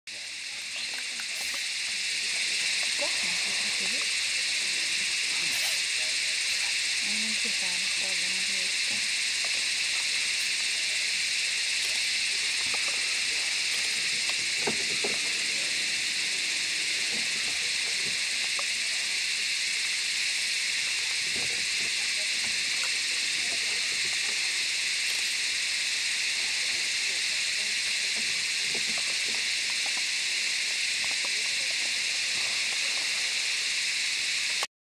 Proboscis monkeys eating in a tree
Two proboscis monkeys eating in a tree in a forest on Borneo. A male and a female. You can tell the difference by the size of the nose.